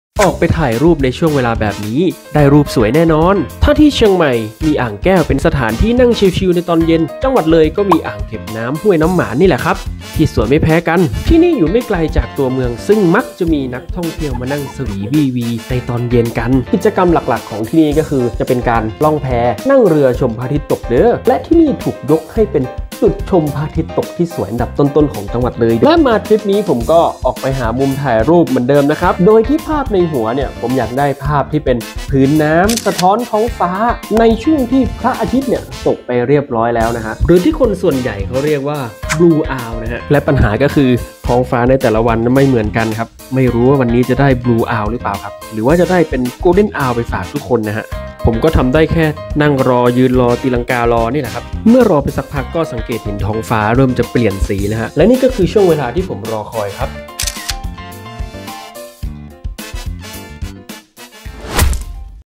ที่เที่ยวเลยหน้าฝน อ่างเก็บน้ำห้วยน้ำหมาน ใกล้ตัวเมือง มุมถ่ายรูปเพียบ 2023
ช่วงเวลาแบบนี้ ถ่ายรูปสวยที่สุด . .
อีกหนึ่งที่เที่ยวหน้าฝน จังหวัดเลย ที่ขับรถออกจากตัวเมืองประมาณ 15 กิโลเมตร ก็ได้เจอสถานที่ท่องเที่ยวธรรมชาติแบบเขียวขจี ทั้งยังเป็นที่บมพระอาทิตย์ตกที่สวยอันดับต้นๆของจังหวัดเลย และถ้าถามว่าเที่ยวเลยช่วงไหนดี? ช่วงฤดูฝนนี่แหละ ชุ่มฉ่ำหัวใจ มาเด้อ .

📍 อ่างเก็บน้ำห้วยน้ำหมาน,เลย